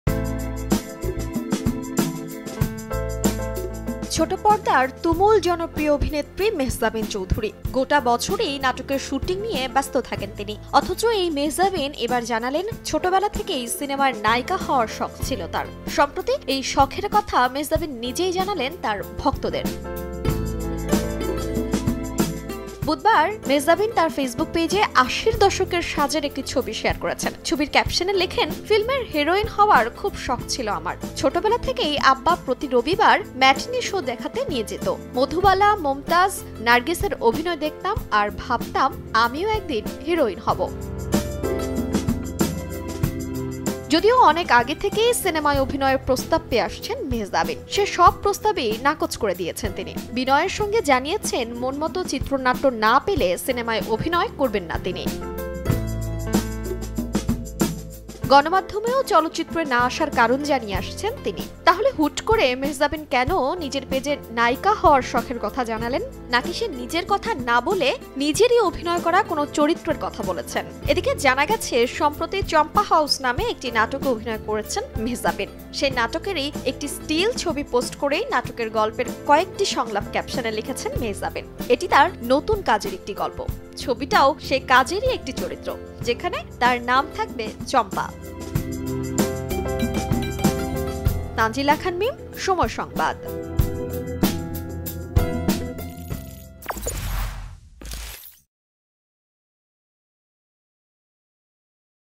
সিনেমার নায়িকা হতে চান মেহজাবিন? | Mehazabien Chowdhury | Bangladeshi actress | Somoy Entertainment
ছোট পর্দার তুমুল জনপ্রিয় অভিনেত্রী মেহজাবিন চৌধুরী।গোটা বছরই নাটকের শুটিং নিয়ে ব্যস্ত থাকেন তিনি। অথচ এই মেহজাবিন এবার জানালেন ছোটবেলা থেকেই সিনেমার নায়িকা হওয়ার শখ ছিলো তার।সম্প্রতি এই শখের কথা মেহজাবিন নিজেই জানালেন তার ভক্তদের।